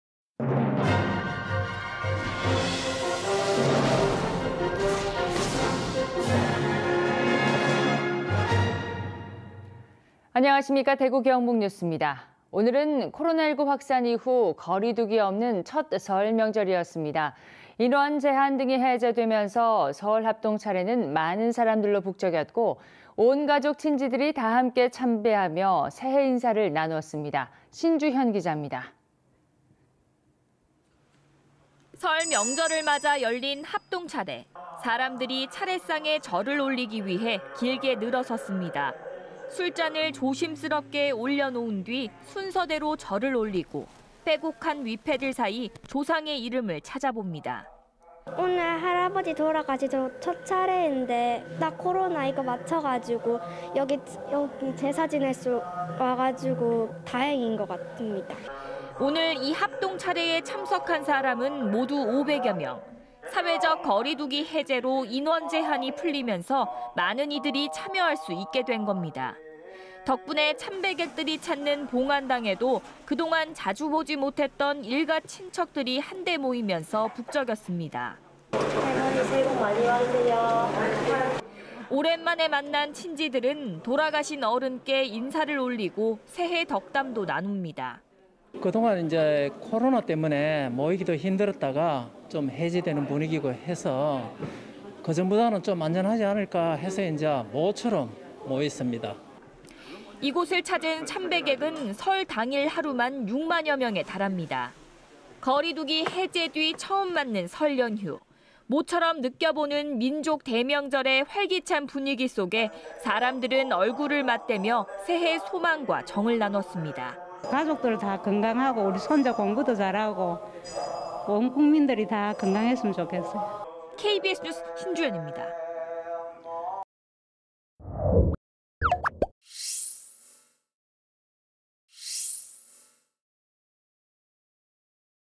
첫 대면 설 ‘반가운 얼굴들’…친지 모여 ‘북적’ / KBS 2023.01.22.
[앵커]

 오늘은 코로나19 확산 이후, 거리두기 없는 첫 설 명절이었는데요.

 인원 제한 등이 해제되면서 설 합동 차례는 많은 사람들로 북적였고, 온 가족 친지들이 다 함께 참배하며 새해 인사를 나눌 수 있었습니다.

 신주현 기자입니다.

 [리포트]

 설 명절을 맞아 열린 합동 차례, 사람들이 차례상에 절을 올리기 위해 길게 늘어 섰습니다.

 술잔을 조심스럽게 올려 놓은 뒤, 순서대로 절을 올리고, 빼곡한 위패들 사이 조상의 이름을 찾아봅니다.

 [박채빈/대구시 수성동 : "오늘 할아버지 돌아가시고 첫 차례인데 딱 코로나 (거리두기 해제)에 맞춰서 제사 지내러 와서 다행인 것 같습니다."]

 이날 이 합동차례에 참석한 사람은 모두 5백여 명.

 사회적 거리두기 해제로 인원 제한이 풀리면서, 많은 이들이 참여할 수 있게 된 겁니다.

 덕분에 참배객들이 찾는 봉안당에도  그동안 자주 보지 못했던 일가 친적들이 한 데 모이면서 북적였습니다.

["할머니 새해 복 많이 받으세요."]

 오랜만에 만난 친지들은 돌아가신 어른께 인사를 올리고, 새해 덕담도 나눕니다.

 [하규동/대구시 읍내동 : "그동안 코로나 때문에 모이기 힘들었다가 해제된 분위기이고 해서 그 전보다는 좀 안전하지 않을까 해서 모처럼 다 모였습니다."]

 이곳을 찾은 참배객은 설 당일 하루만 6만여 명에 달합니다.

 거리두기 해제 뒤 처음 맞는 설 연휴.

 모처럼 느껴보는 민족 대명절의 활기찬 분위기 속에 사람들은 얼굴을 맞대며 새해 소망과 정을 나눴습니다.

 [이정숙/대구시 다사읍 : "가족들 다 건강하고 우리 손자 공부도 잘하고 온 국민들이 다 건강했으면 좋겠어요."]

 KBS 뉴스 신주현입니다.

 촬영기자:최동희